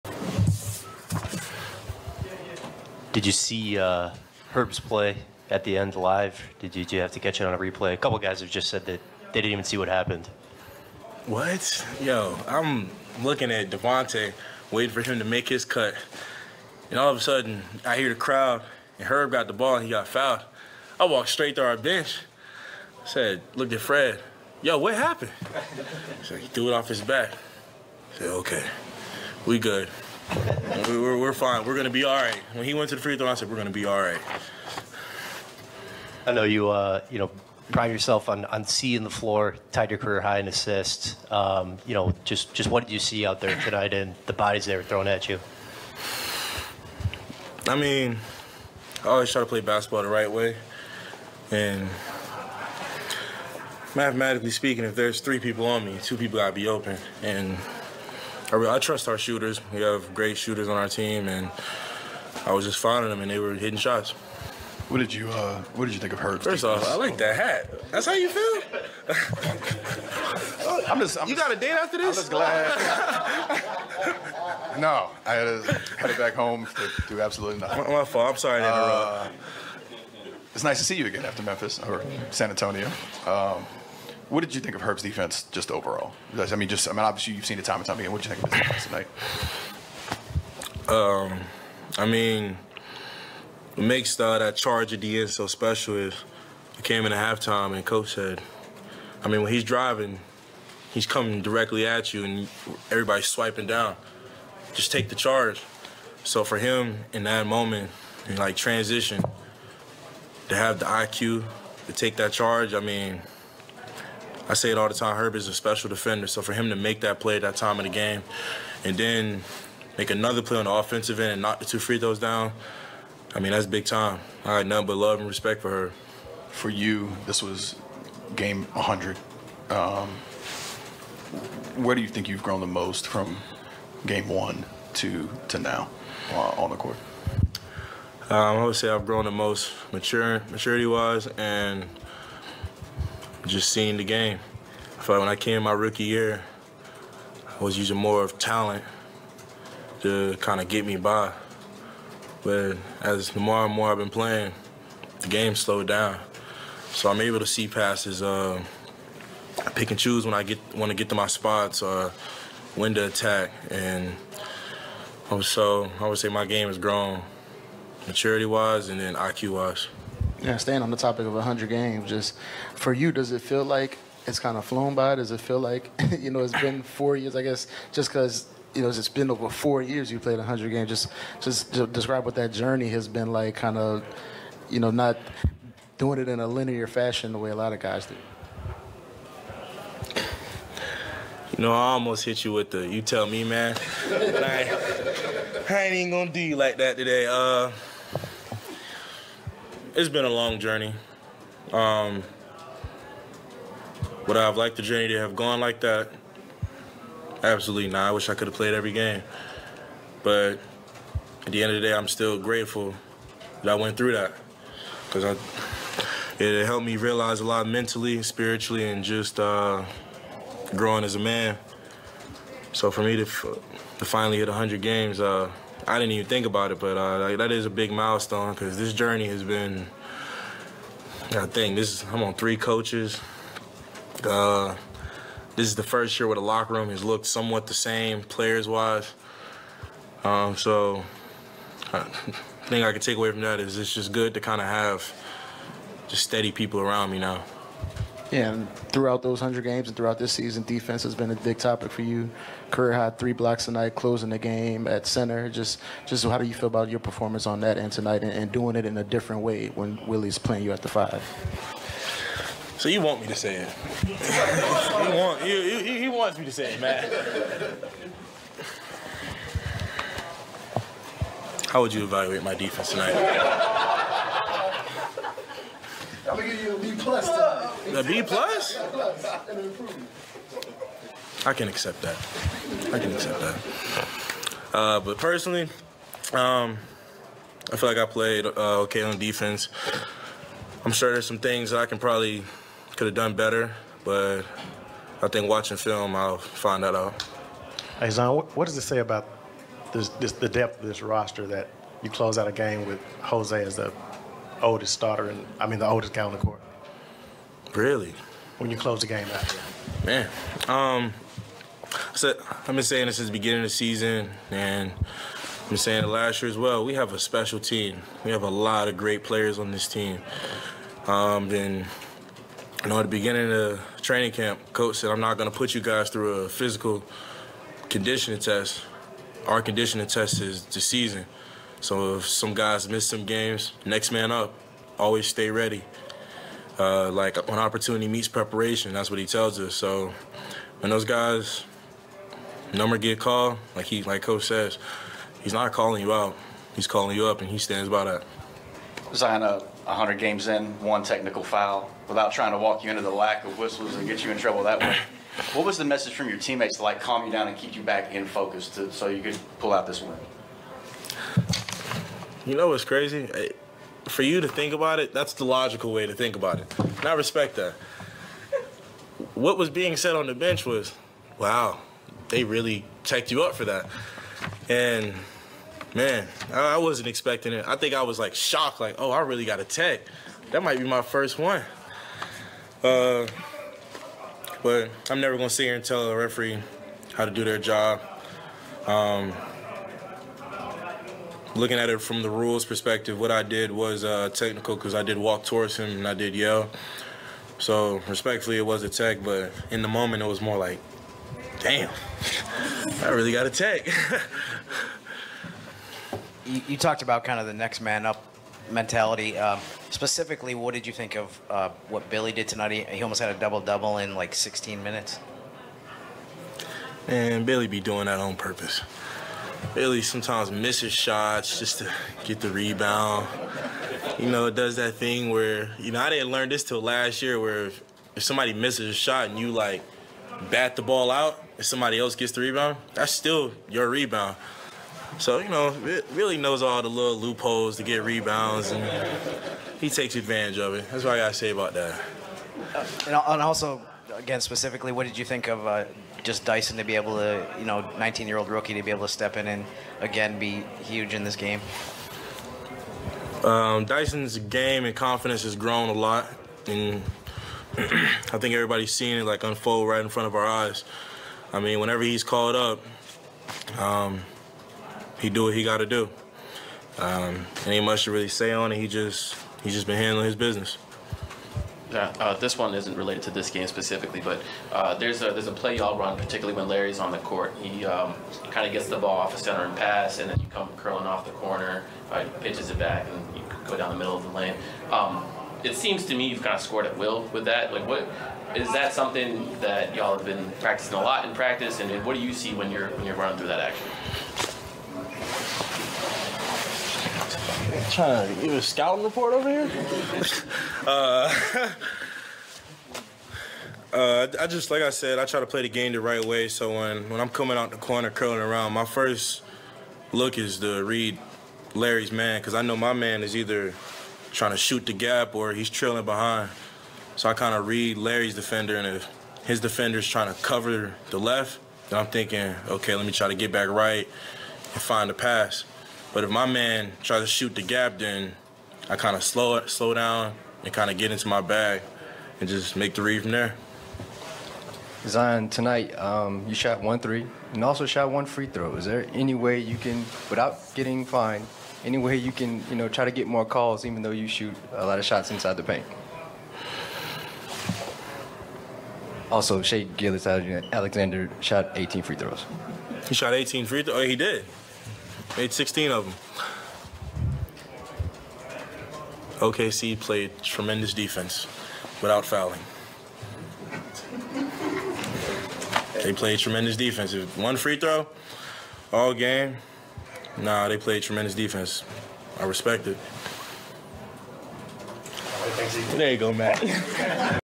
Zion Williamson postgame interview NOP 105, OKC 101
New Orleans Pelicans Zion WIlliamson postgame interview after win vs Oklahoma CIty Thunder 105-101